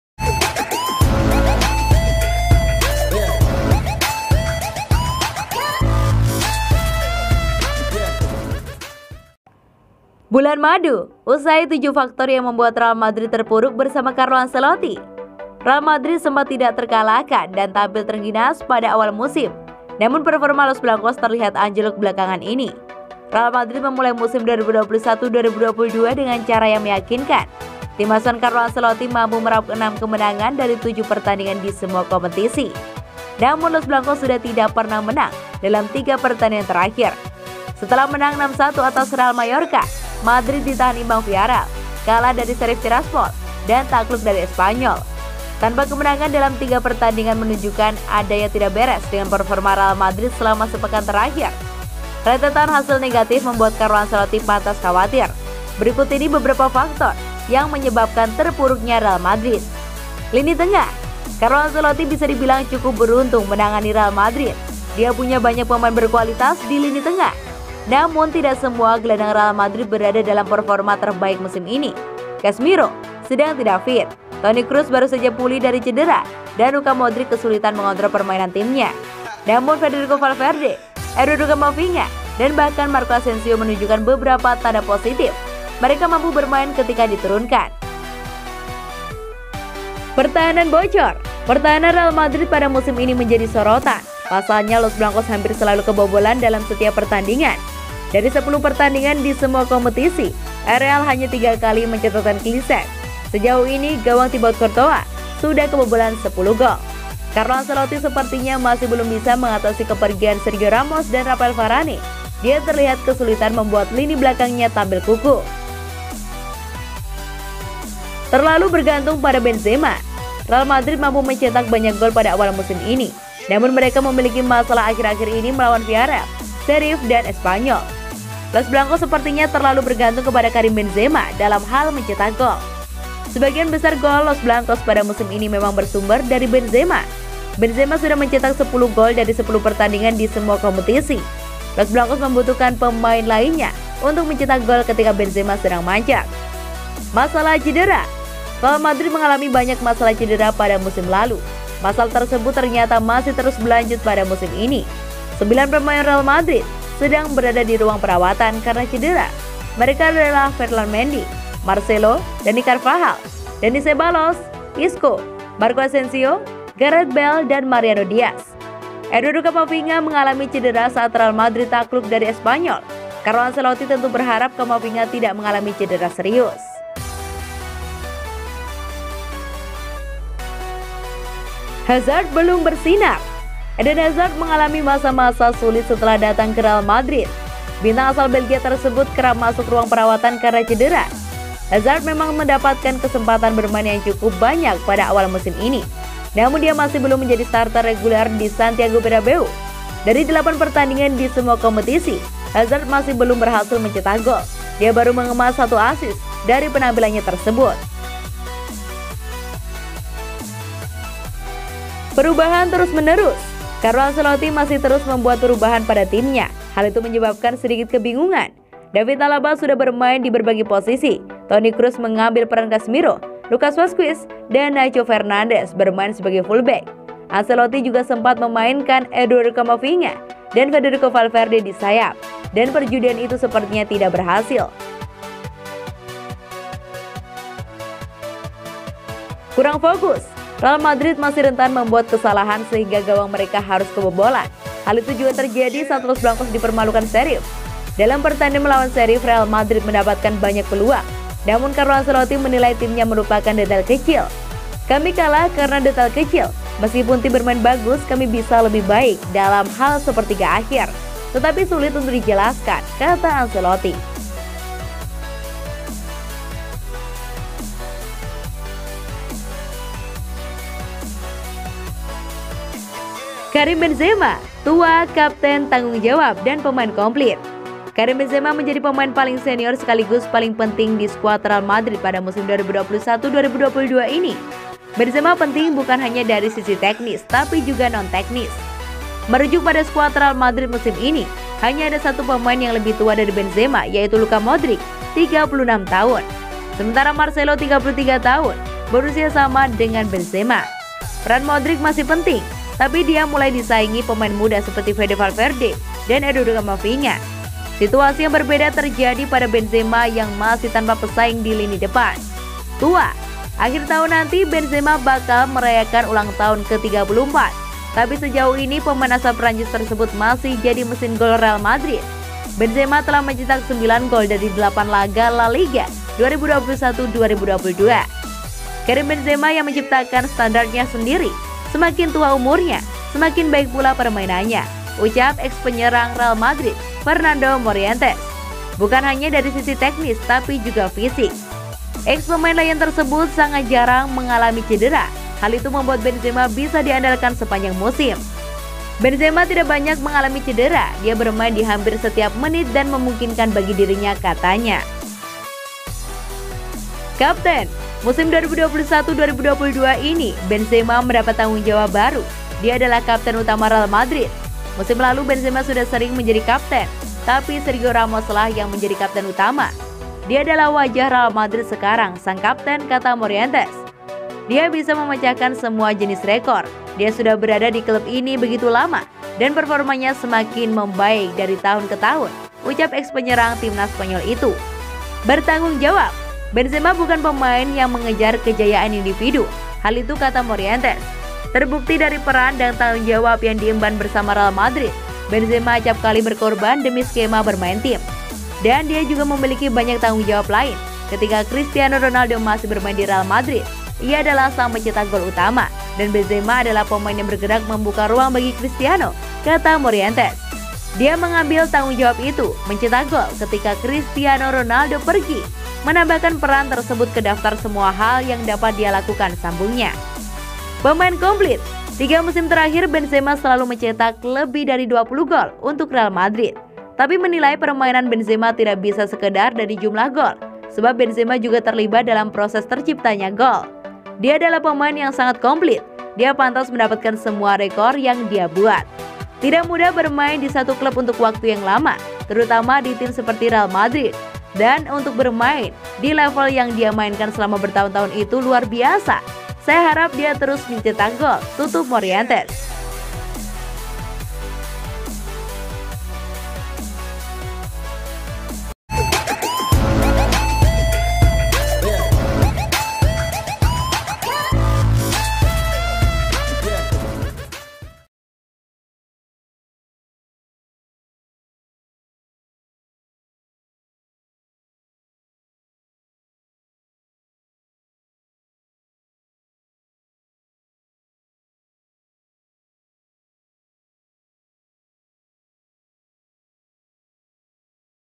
AYO BANGKIT‼️7 Faktor Yang Buat Madrid Terpuruk 🧐 Benzema Pemain Paket Komplit 🔥
Isi Berita Real Madrid Hari ini:

- ‘Bulan Madu’ Usai, 7 Faktor yang Membuat Real Madrid Terpuruk Bersama Carlo Ancelotti
- Karim Benzema: Tua, Kapten, Tanggung jawab, dan Pemain Komplet